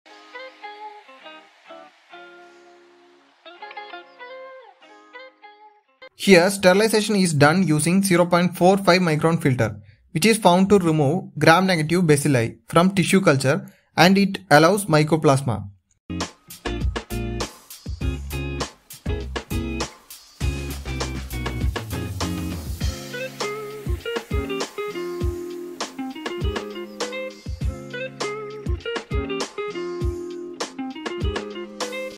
Sterilization with a 0.45-micron filter effectively removes gram-negative bacilli from tissue cultu…
Sterilization with a 0.45-micron filter effectively removes gram-negative bacilli from tissue culture media while mycoplasmas pass through the filter. Which of the following microbial structures most likely accounts for this difference? Capsule Cytoplasmic membrane DNA Peptidoglycan Ribosome